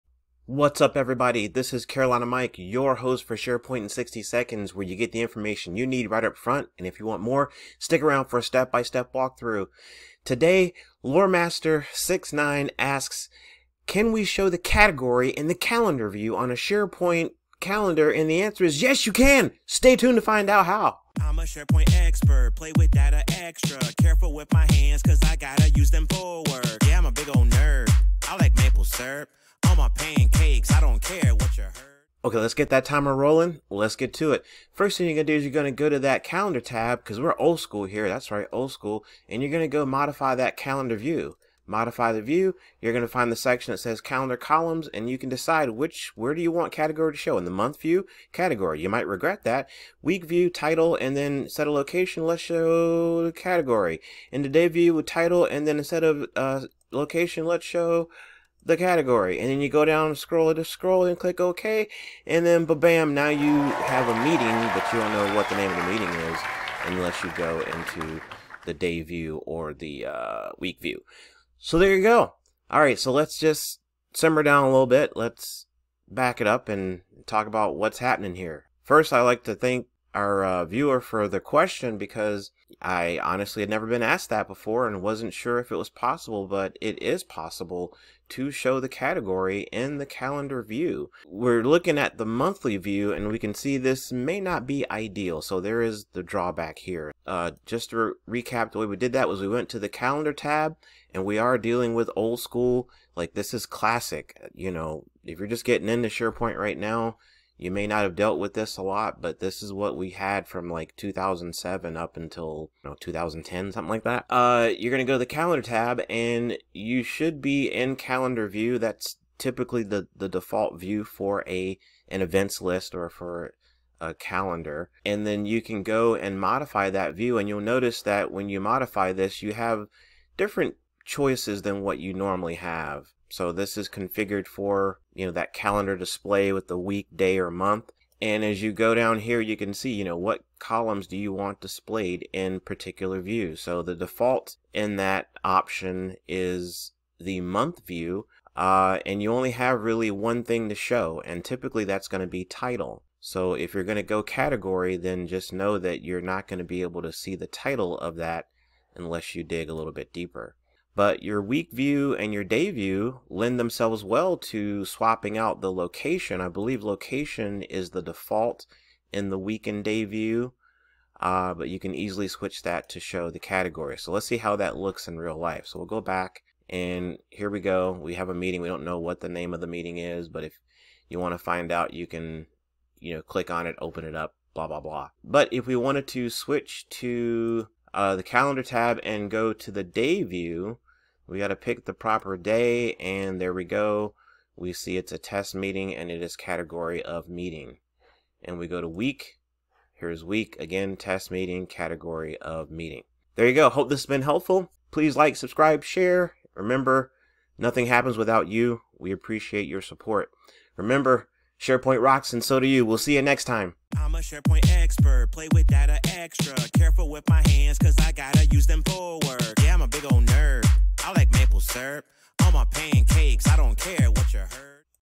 SharePoint: Show Category In Classic Calendar and Events List Views
Watch to see how easy it is to add the category to classic calendar views in SharePoint!